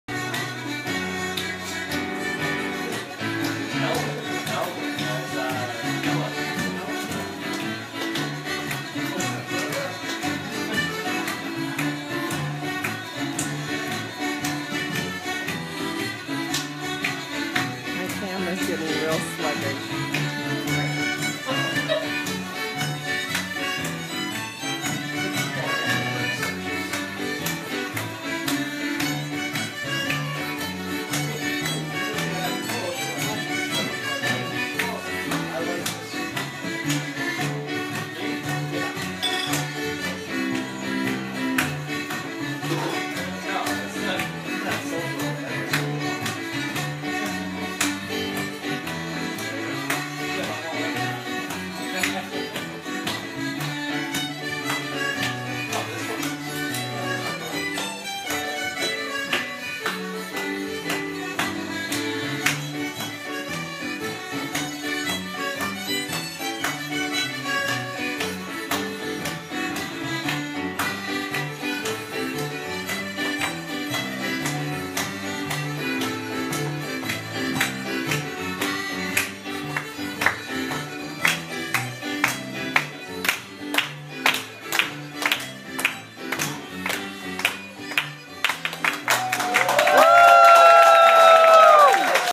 Katie & O'Hare Irish Dancers at Lake Bluff Brewing Company St. Patrick's Day 2013
Katie & several other Irish Dancers from O'Hare School of Irish Dance, Lake Bluff performed at the Lake Bluff Brewing Company on St. Patrick's Day 2013